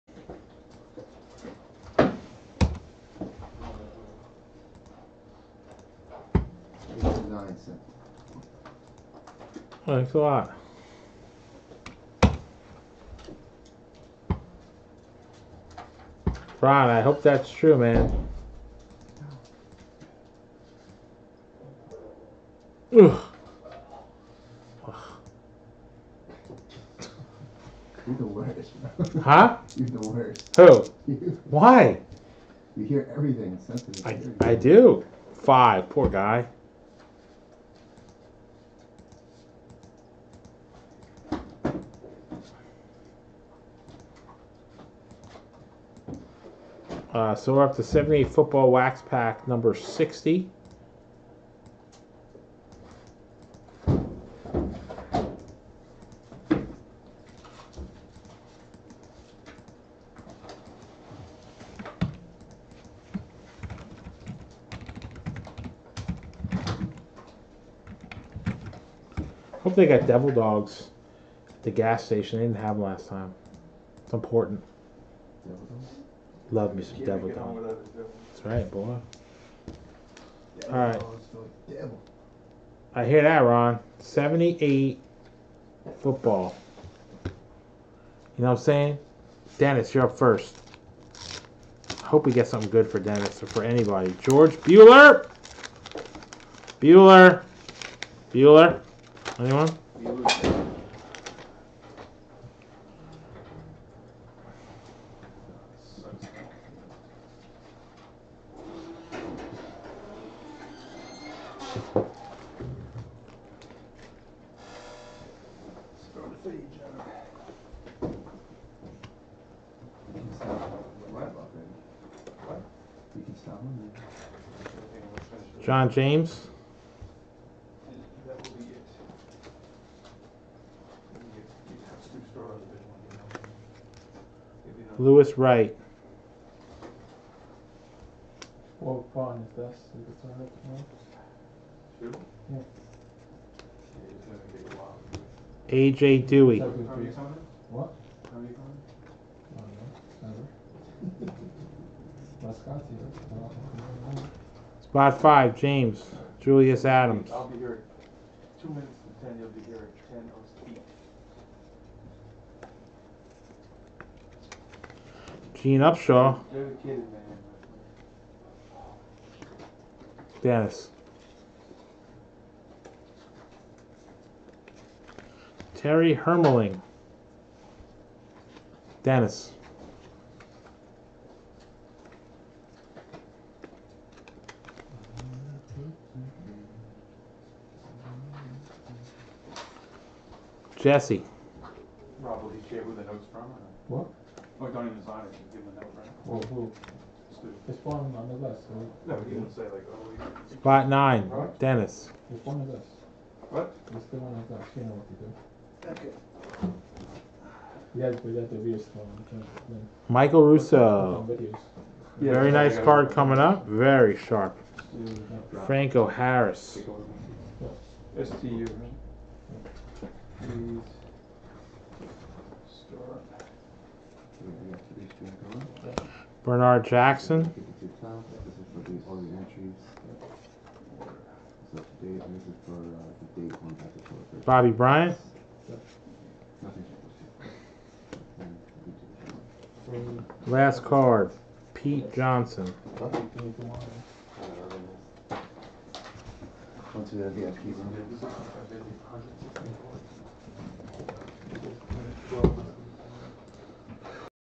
04_10_18 - 1978 Topps Football Wax Pack Break Video Opening
Tonight we opened a 1978 Topps Football Wax Pack.